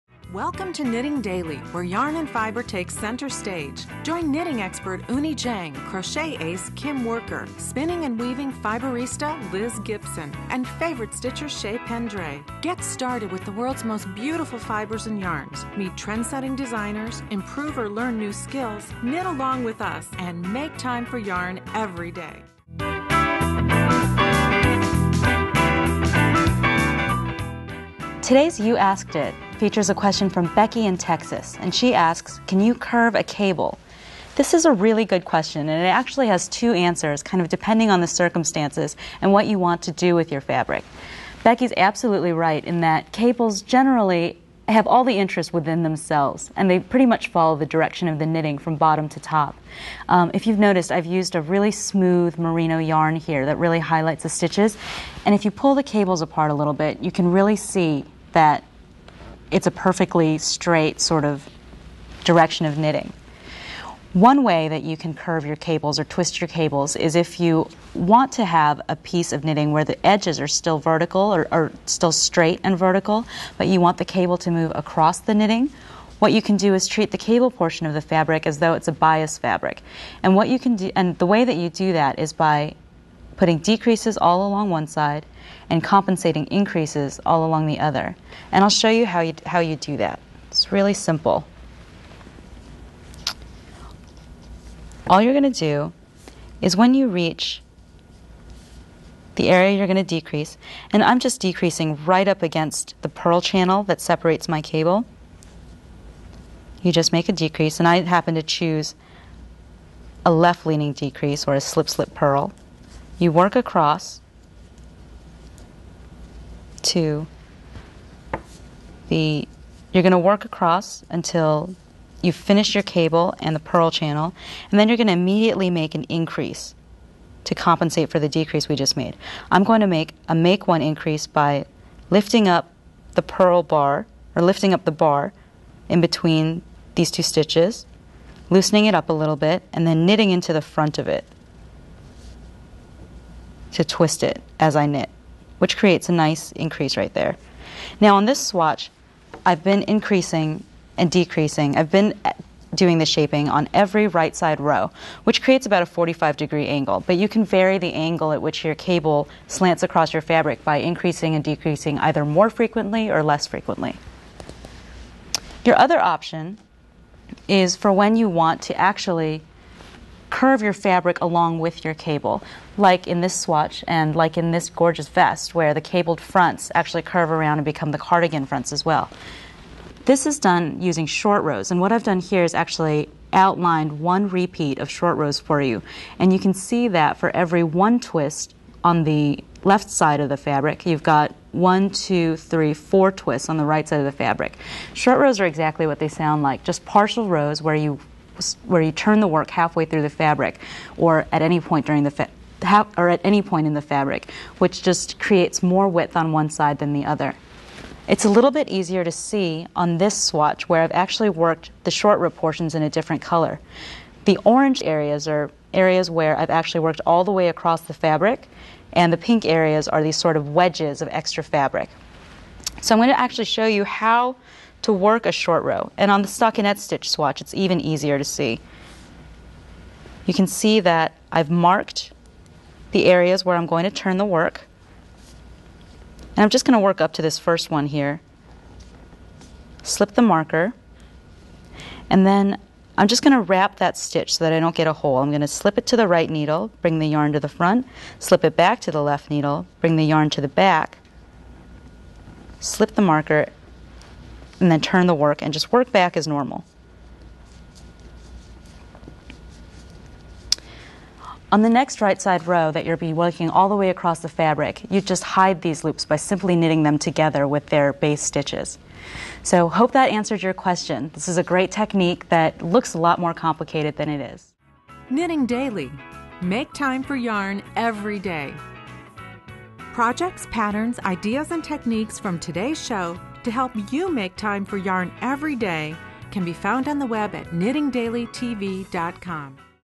How to Knit Cables
Eunny Jang investigates knitting cables in this exclusive from knittingdailytv.com